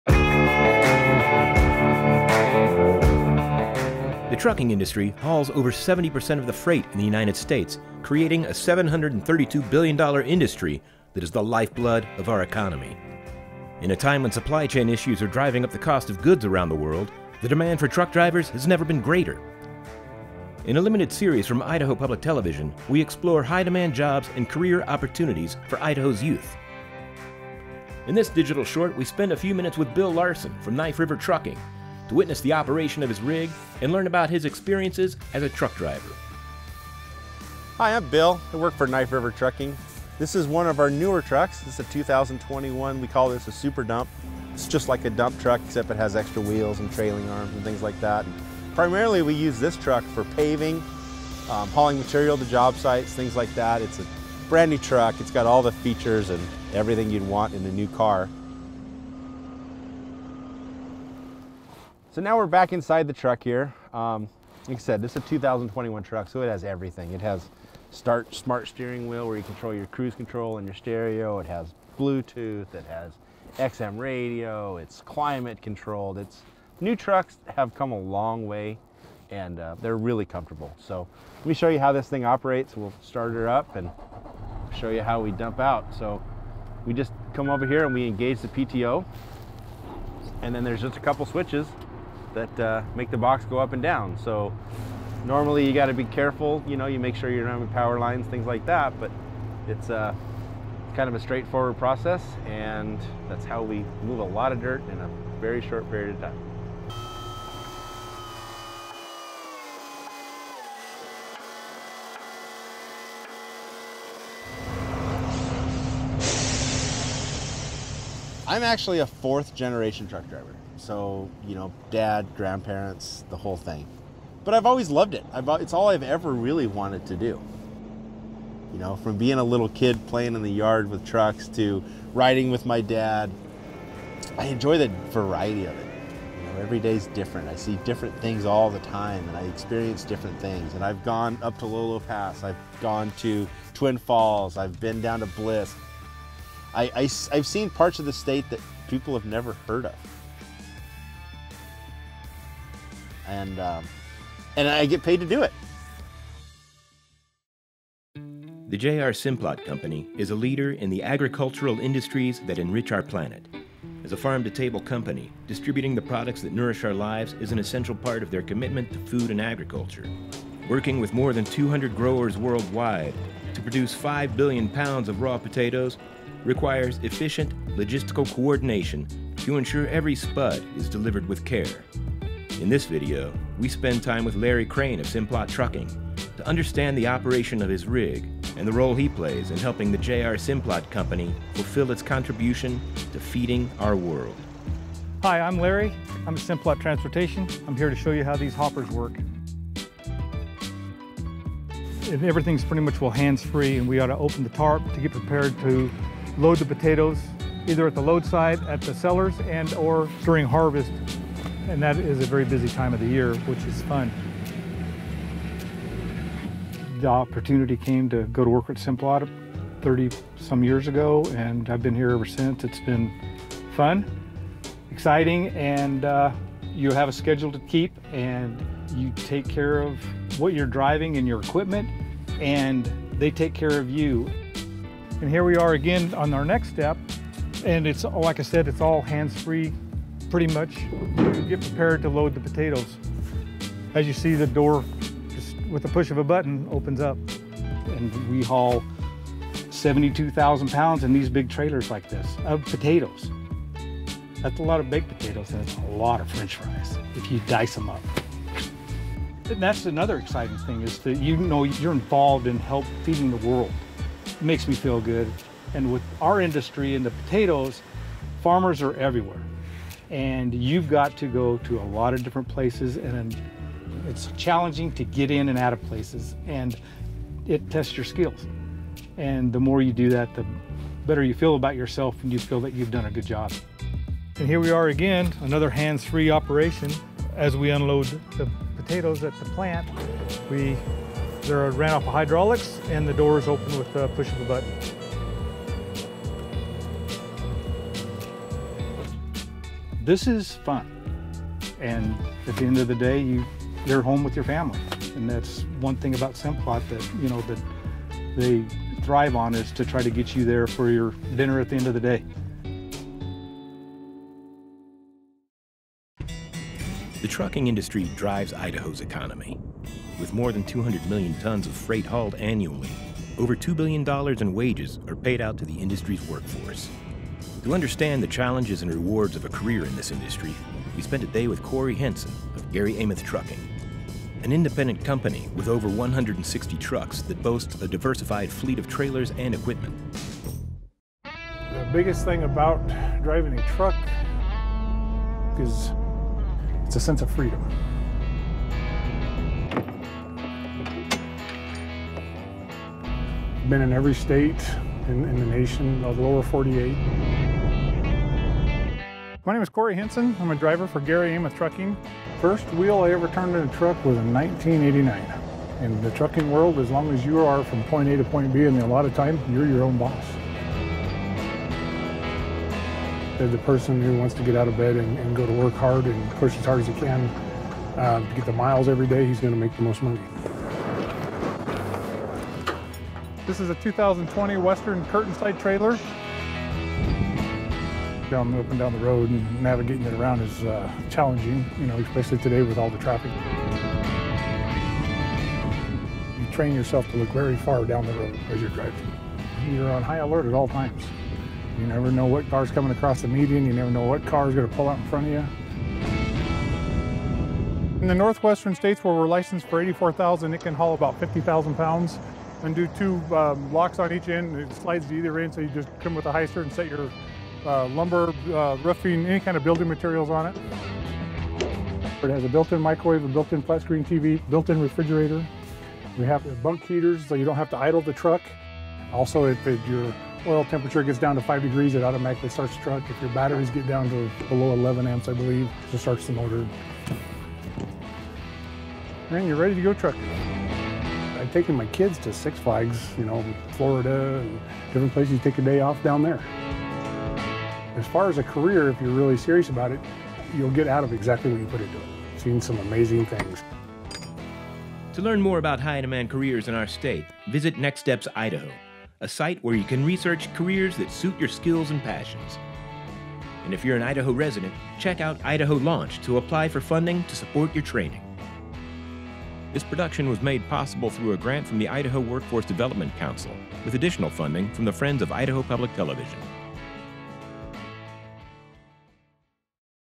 Idaho Trucking Association 360
In this video we explore the various challenges and rewards to a career in the trucking industry.

This is a 360° immersive video and is best viewed with VR goggles.  If viewing on a desktop use your mouse to click and drag and explore the video.  If viewing on a phone or tablet tilt your device in the direction you want to look.  Have fun and enjoy!